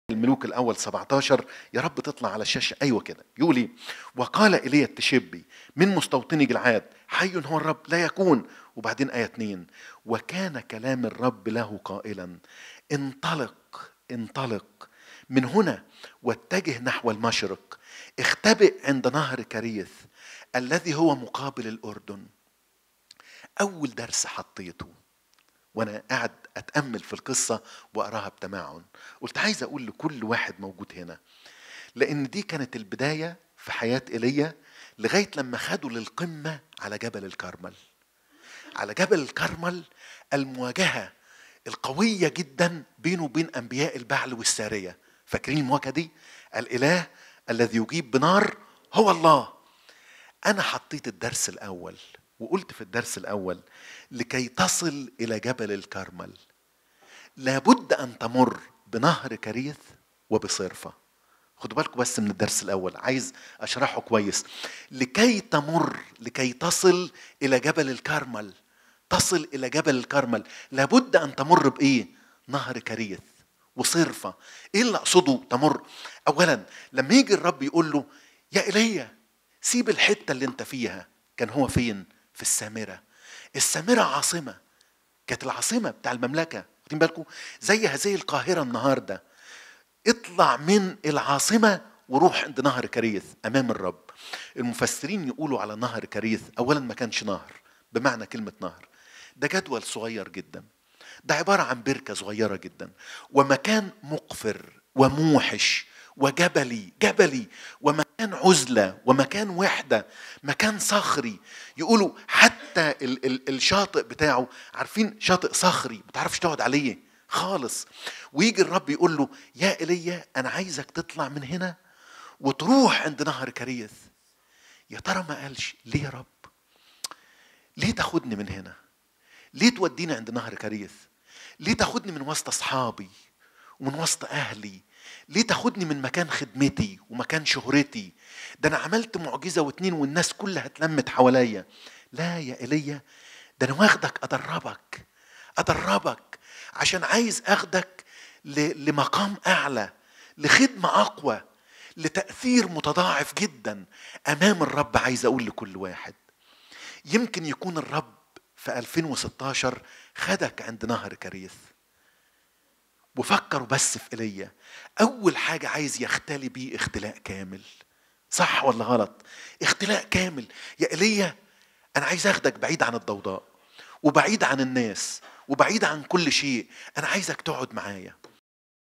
الدرس الأول في دعوة إيليا - القس عزت شاكر - اجتماع الحرية
اجتماع الحرية | سلطان الله - القس عزت شاكر | بث مباشر على المحطة الفضائية سات-7 يوم الاثنين 19 ديسمبر 2016 | انتاج سات-7 | اخراج جون عدلي Rev. Ezzat Shaker - Brethren Church Triumph Square - Freedom Meeting - SAT-7